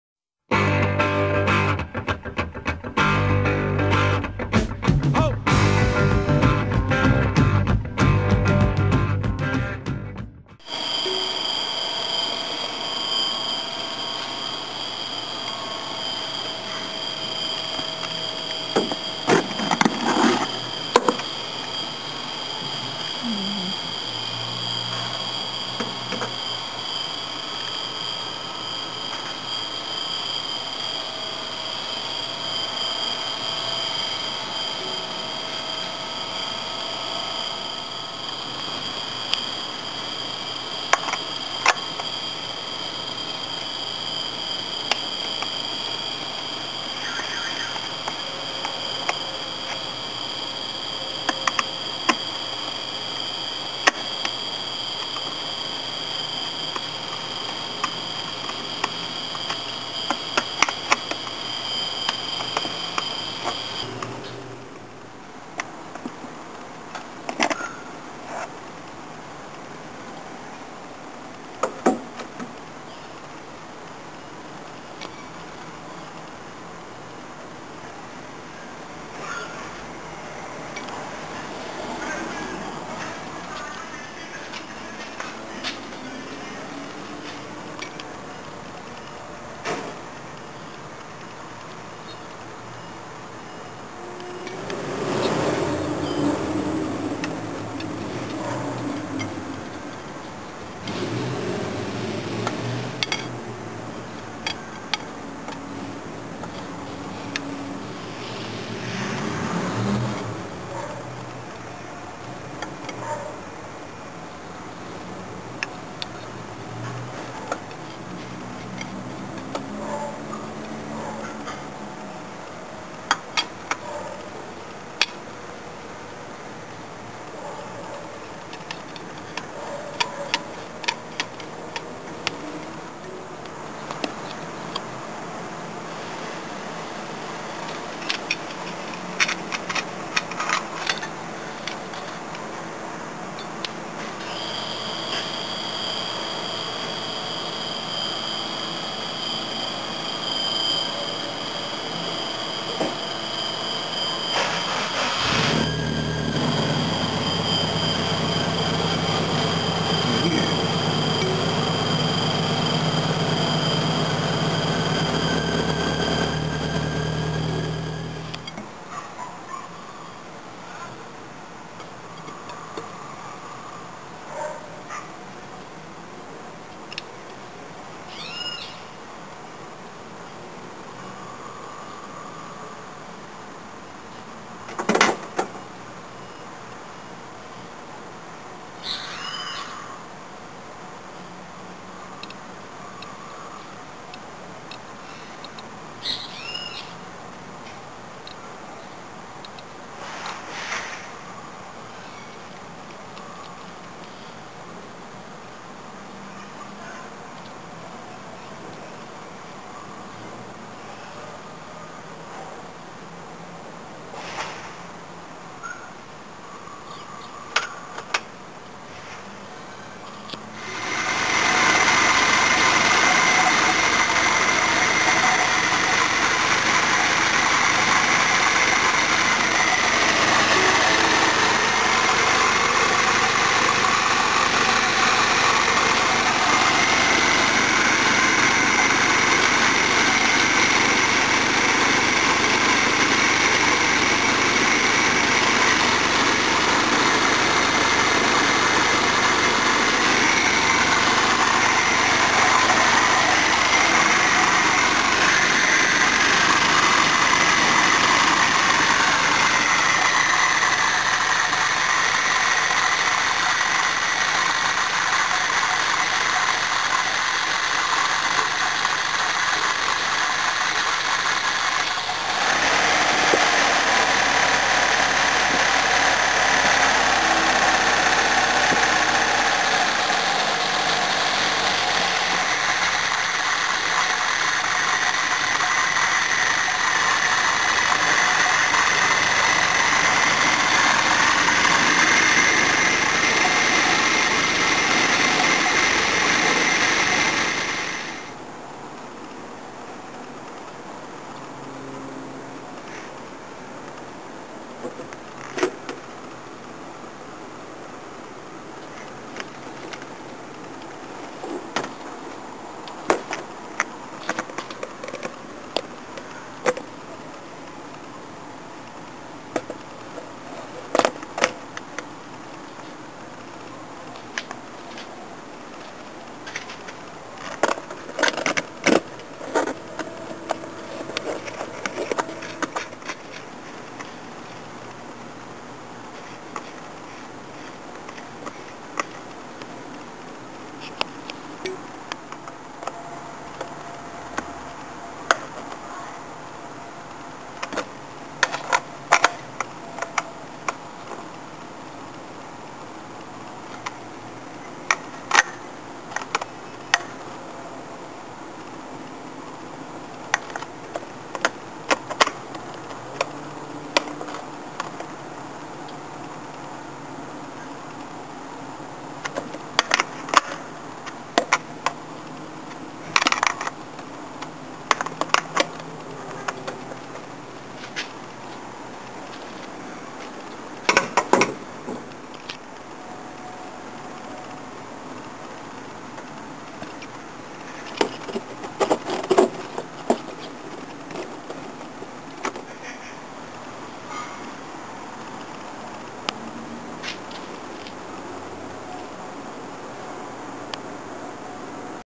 Ford Corrtina MK4 Weber 32/36 service
Took down my Weber 32/36 carby to look at the accelerator pump check valve. Its a 1.6 and originally had the single barrel 1.6 carb so I had to do some modding to the original 1.6 air box for it to fit the Weber. The 1.6 throttle linkage was also too long so I use a makeshift adapter plate to get it to line up properly with the Weber.